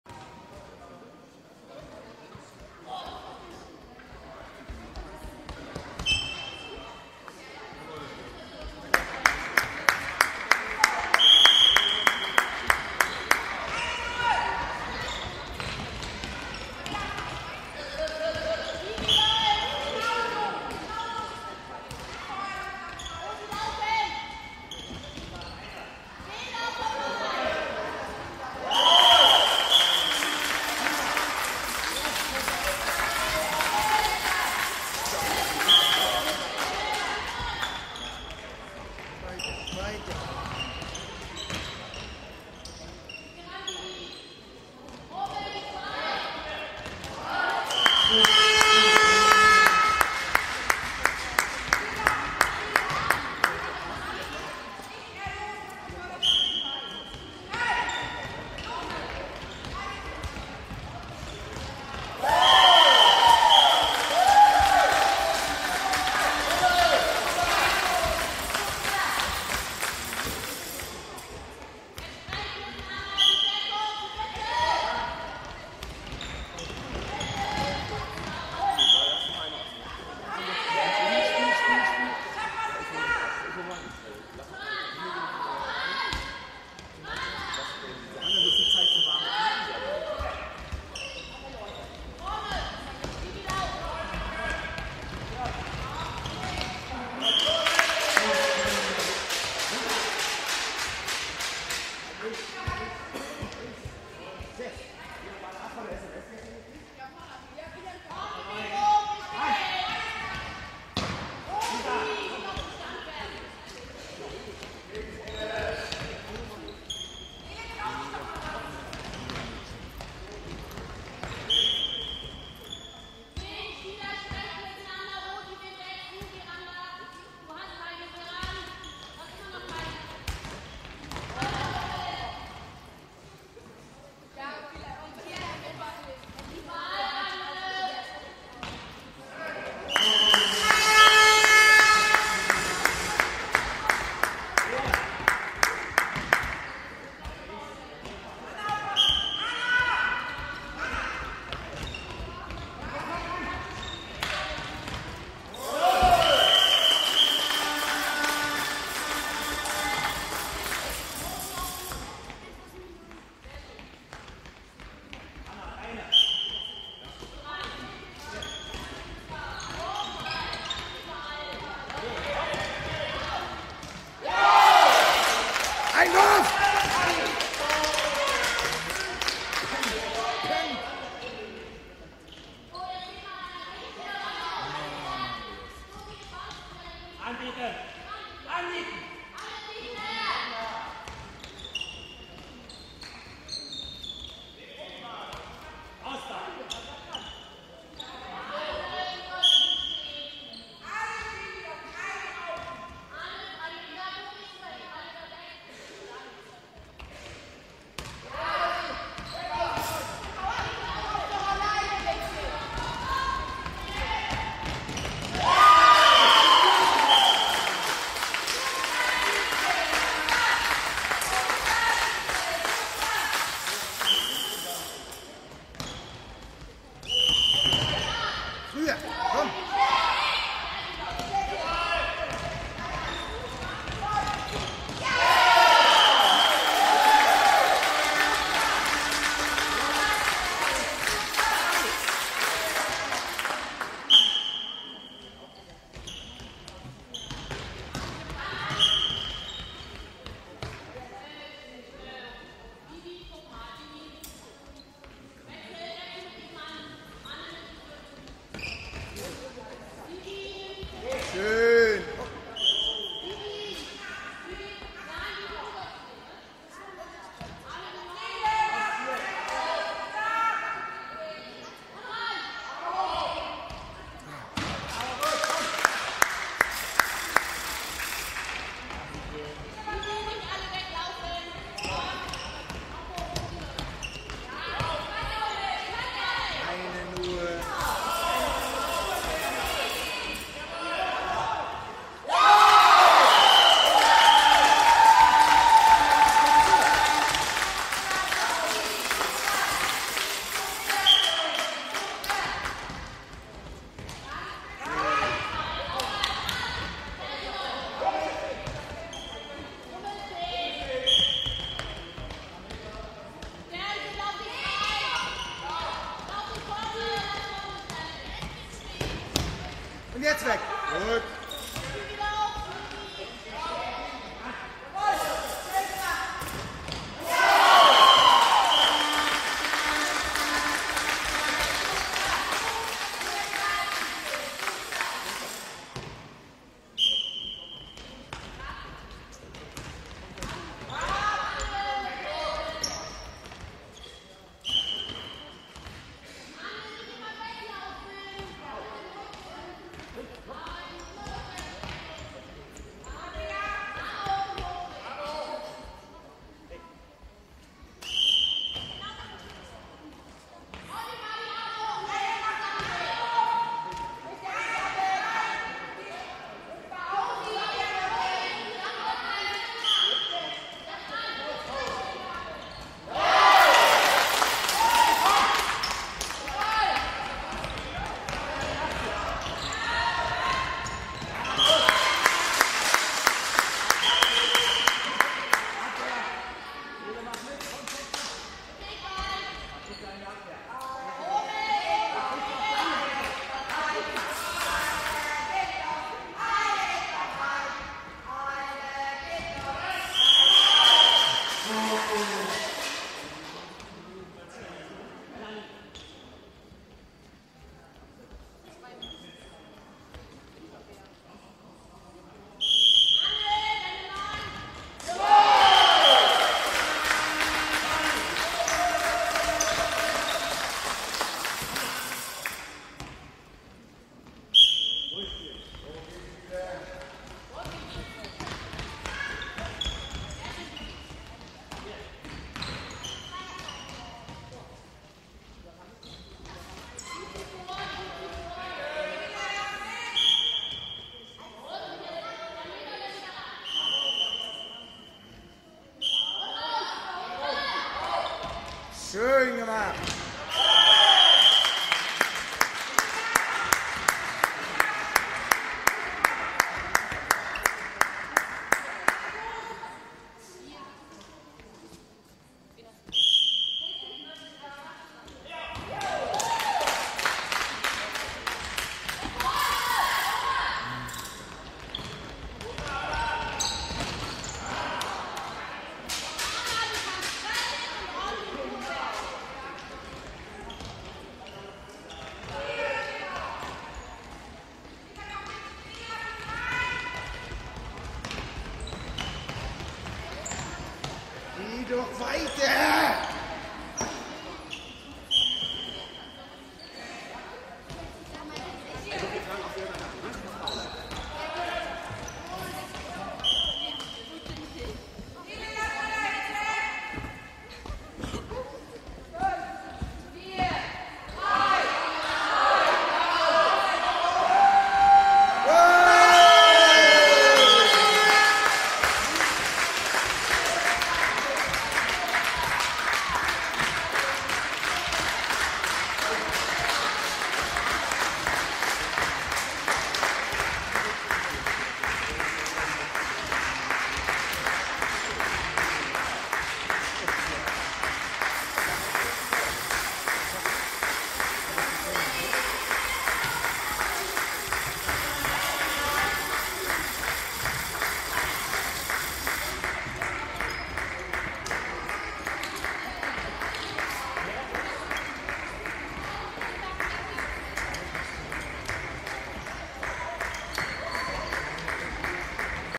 Sport inSchwerin 34s1 T 05
Handball Spielunion wJE Sportverein Grün-Weiß 10:30 (3:15) SV GW Schwerin II vs. VfL BW Neukloster